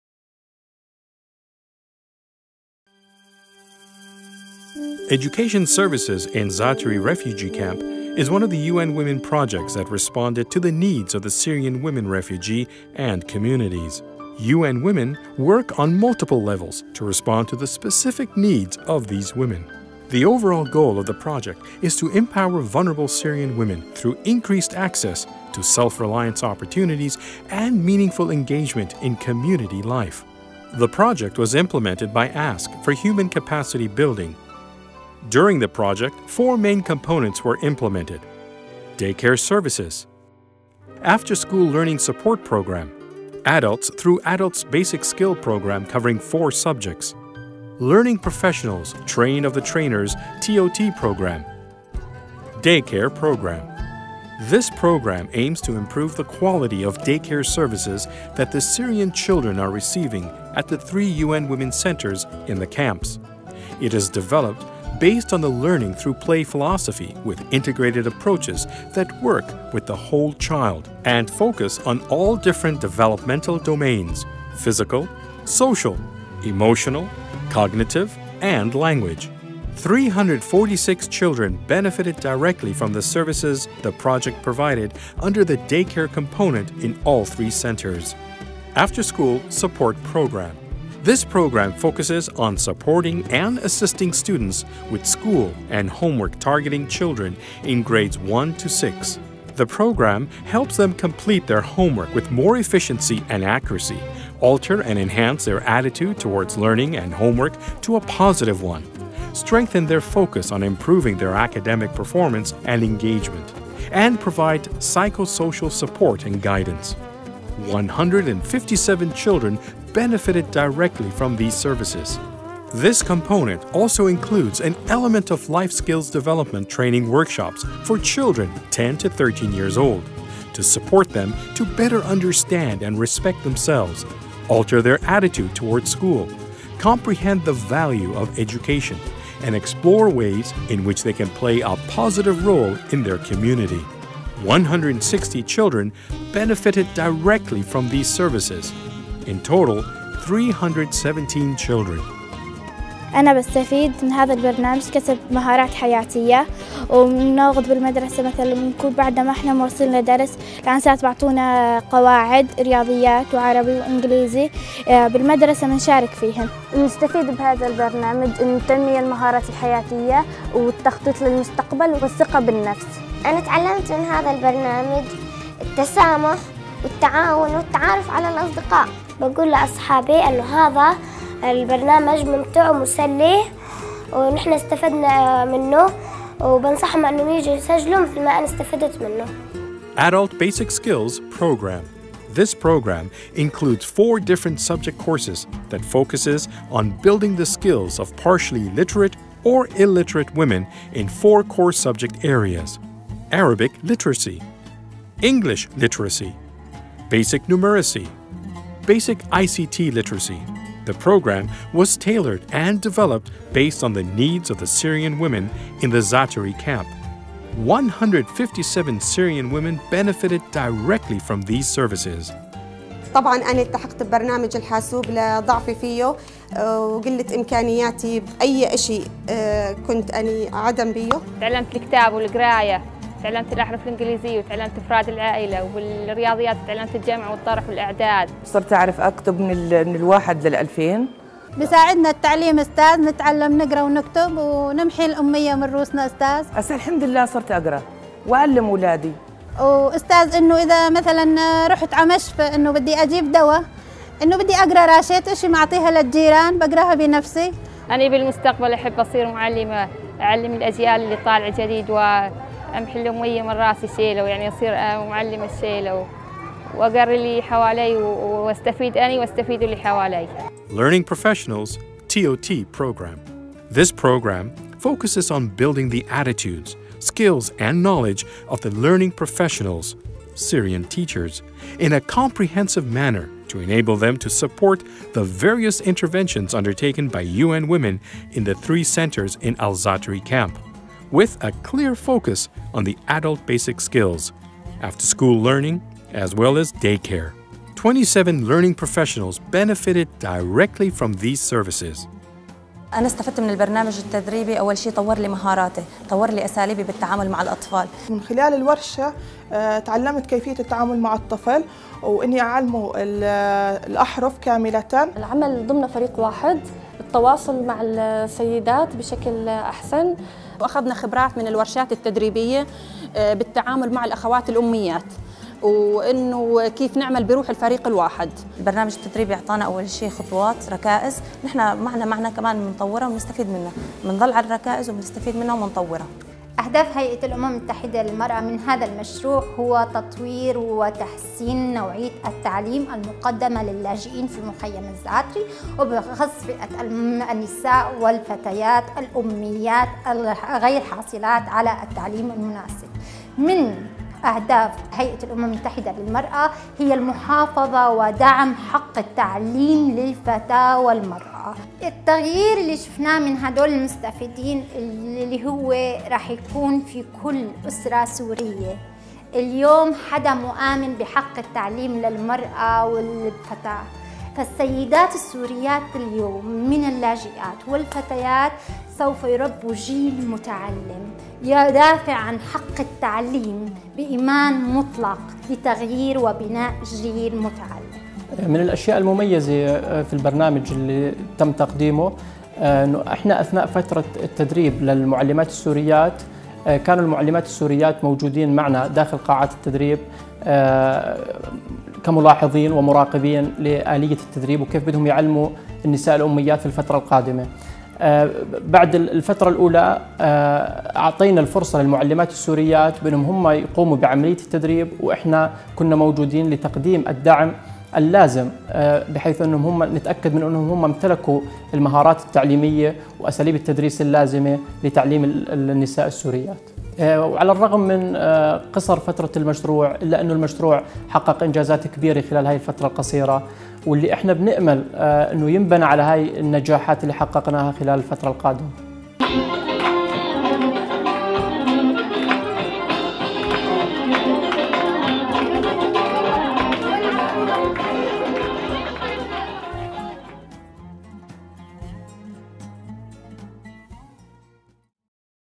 UN Women project at the Za’atari Refugee Camp - ASK for Human Capacity Building
The project aims at improving the quality of educational services provided at 3 centers in Za’atari Camp that target illiterate women, children ages (6-12 years), children in kindergarten, in addition to the Syrian teachers who provide these services at the centers. To achieve the set goal, ASK will study the needs of the target group and develop a training program that targets 100 Syrian woman, 80 Syrian children, 100 kindergarten children and 30 Syrian teachers who were hired by UN- Women to offer services to Syrian refugees at the camp. The training program will consist of 3 components which are:

[A] Developing the competencies of Syrian teachers program: targets (30) Syrian teachers in 3 centers. The program is focused on equipping them with the needed teaching skills and competencies to work with adult learners as well as kindergarten aged children simultaneously, in addition to promoting a sense of responsibility in children.

[B] Literacy Program: targets (100) illiterate woman. The program is specialized in teaching Arabic language, English language, mathematics, and computer.

[C] Children’s program for ages (6-12 years): targets (80) children. The program focuses on providing the necessary support for children to complete their homework together and develop responsibility and self-confidence skills … etc.

ASK’s expert consultants will implement the training program according to the latest best training strategies that commensurate with the target group and the difficult circumstances they are undergoing. We are full of hope for a successful implementation to achieve the project’s noble goals, reduce the suffering of our Syrian brothers and sisters, and add this project to ASK’s success stories in the field of Education and building human capital.